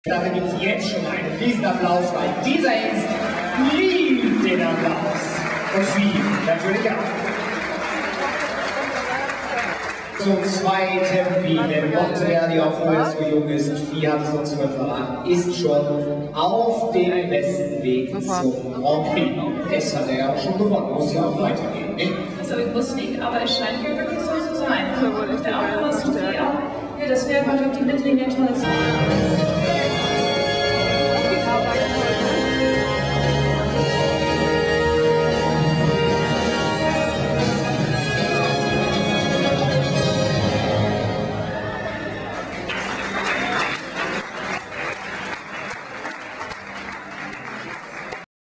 EH Monteverdi
Trakehner stallion EH Monteverdi TSF (by EH Partout - Matador) during the Gala in Leverkusen 2003 with Fie Skarsoe.